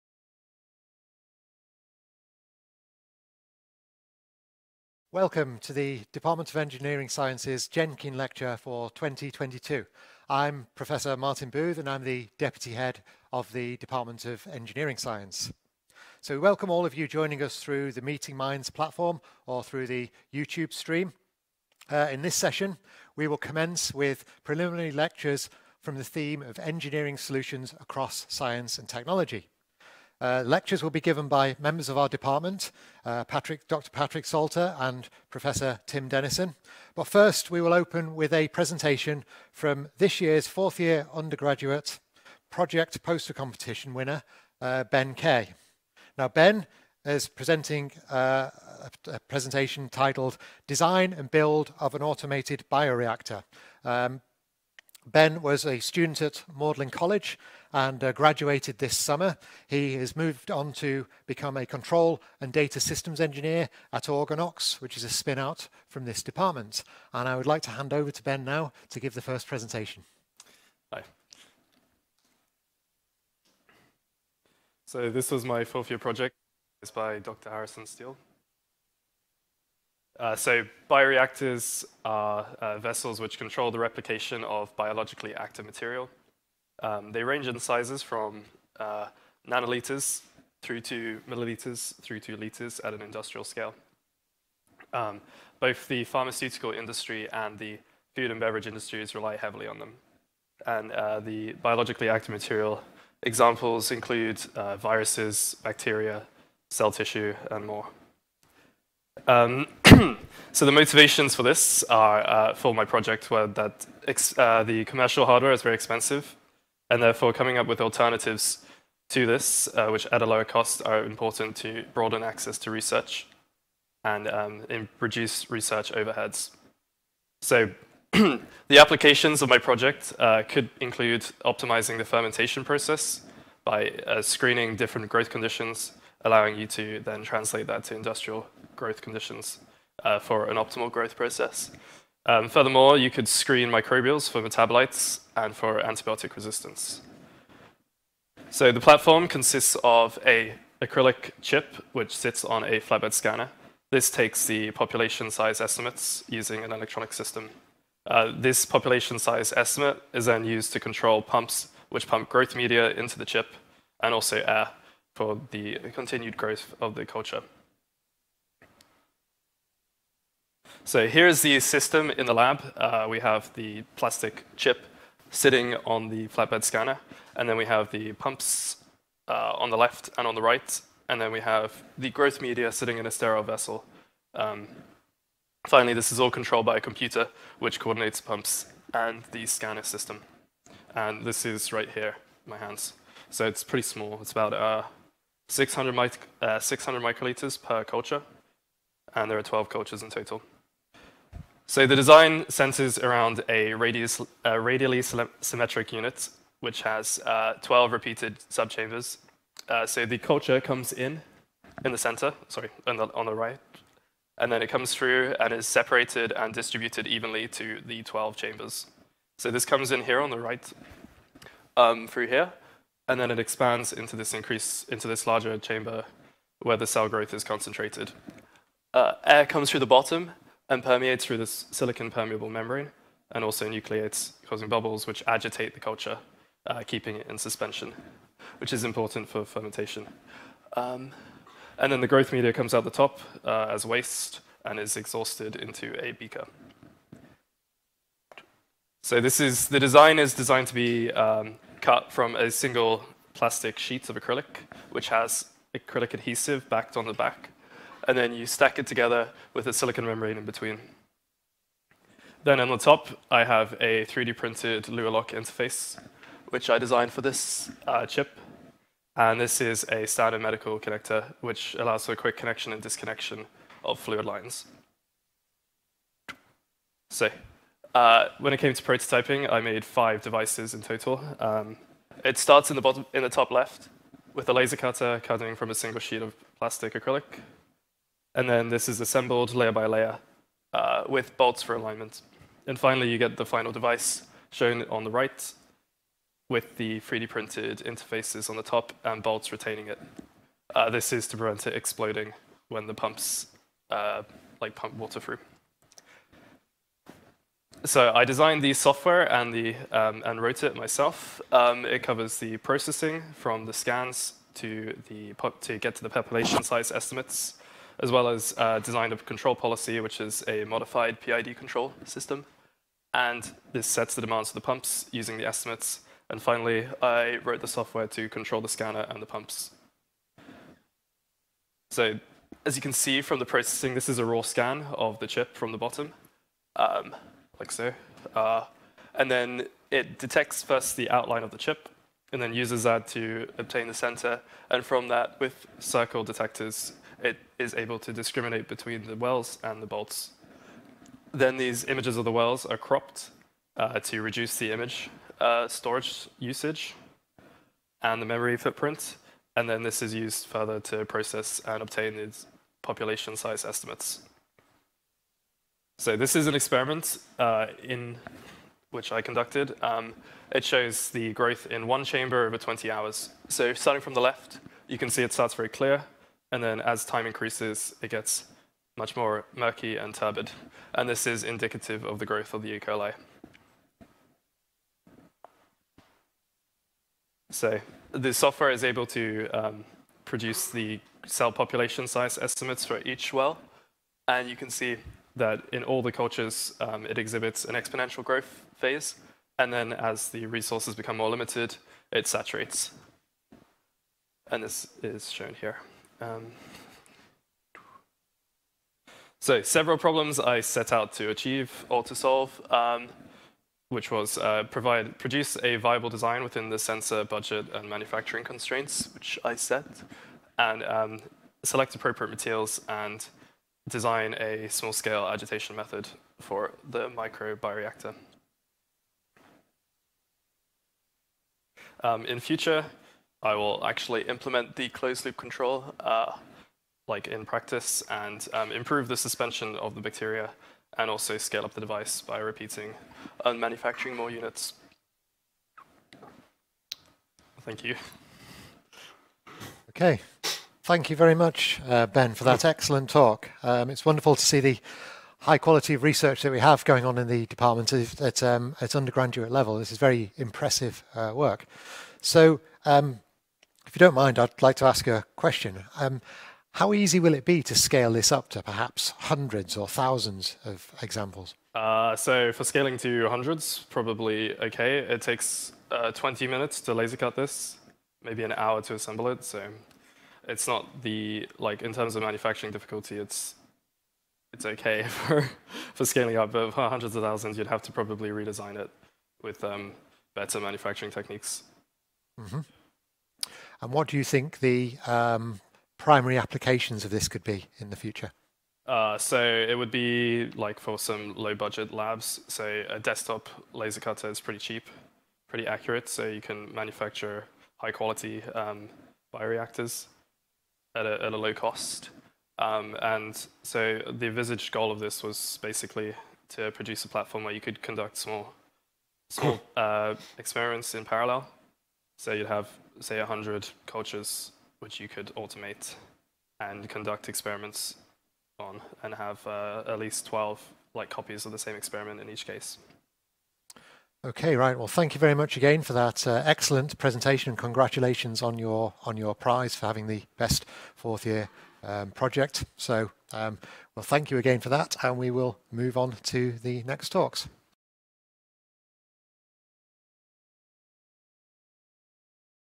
35th Jenkin lecture 17 Sept 2022: 4th year undergraduate project poster winner talk
The 4th Year Project Poster competition has been running since 2001 and continues to provide our undergraduate students with the opportunity to write and present their first academic poster.

The 4th Year Project Poster Competition is an amazing platform that offers University of Oxford - Department of Engineering Science 4th Year students the opportunity to showcase their work to representatives from industry, alumni and researchers/academics in the Department of Engineering Science.

Based on the projects completed by all 4th year undergraduate students, students are invited to present a poster on their work to a panel of judges with the aim to winning one of four categories or being the overall winner and a cash prize. The overall winner received not only a further cash prize but also the opportunity to present their project at the Department of Engineering Science Jenkin Lecture in the Autumn. We present this year's lecture by Overall Best Poster winner Ben Kaye.